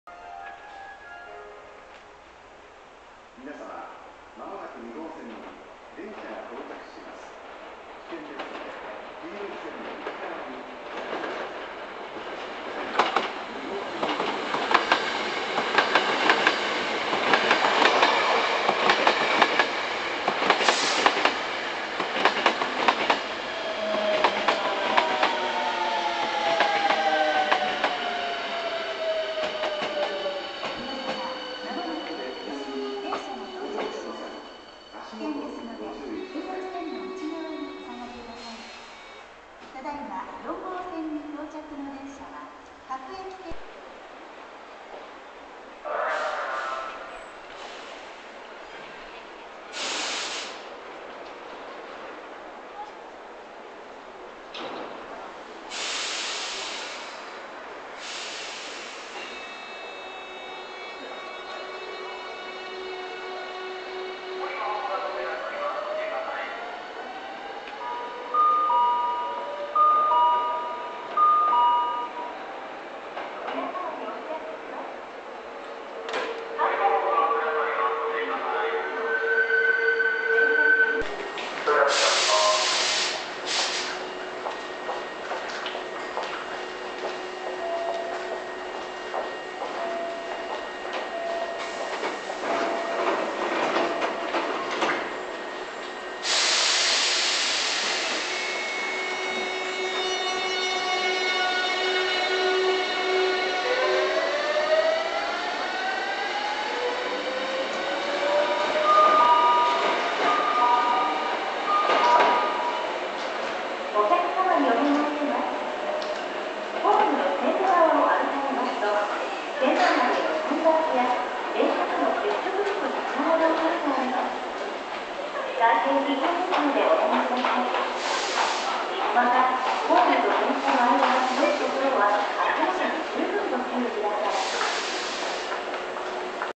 阪急電鉄 8000系 8001F C#8001 東芝SiCインバータ PMSM長期実用試験車 特急 新開地行き 西宮北口駅にて 阪急神戸線
阪急電鉄 8000系 8001F
C#8001 東芝SiCインバータ PMSM長期実用試験車
特急 新開地行き 西宮北口駅にて